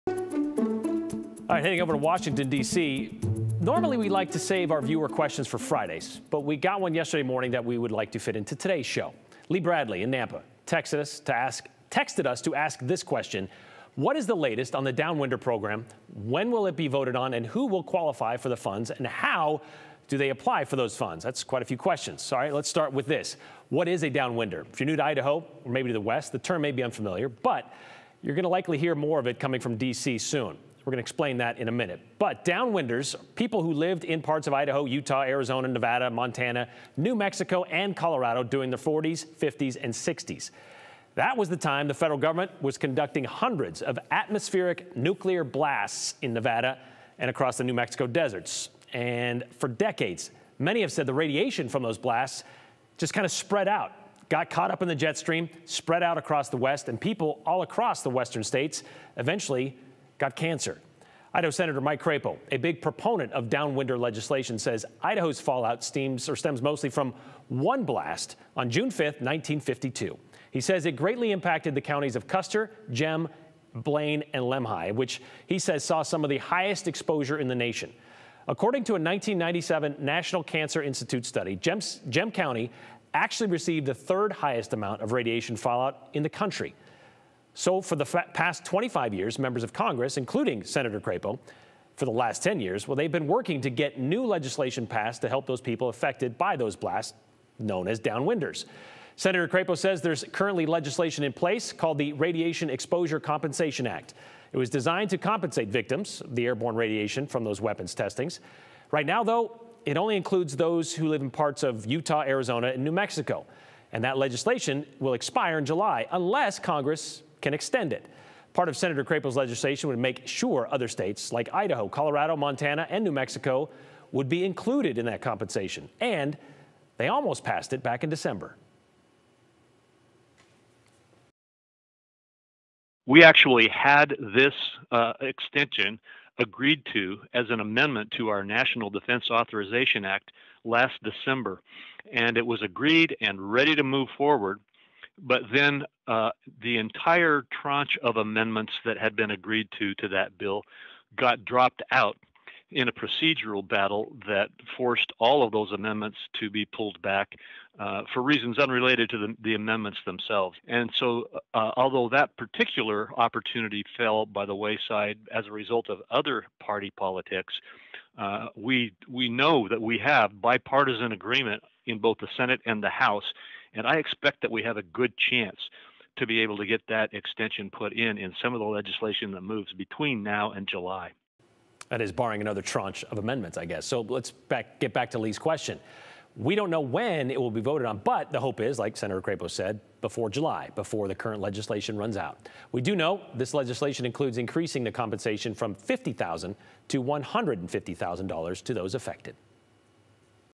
Viewer question: What is the latest on the downwinder program?
The Radiation Exposure Compensation Act is set to expire in July, unless Congress extends it. Sen. Mike Crapo believes there is a "good chance" the extension occurs.